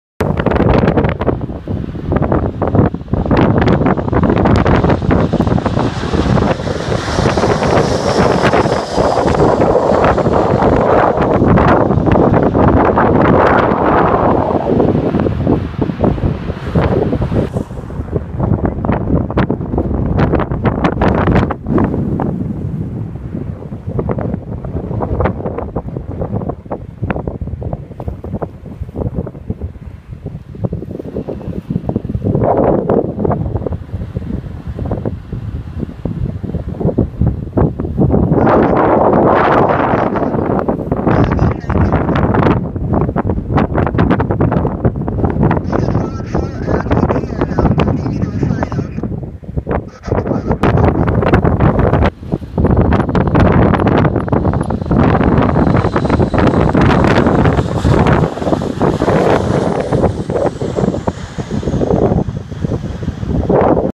LGMK Avro RJ orbit on short final
An AVRO RJ orbit on short final runway 34 Mykonos due to still backtracking previous landing aircraft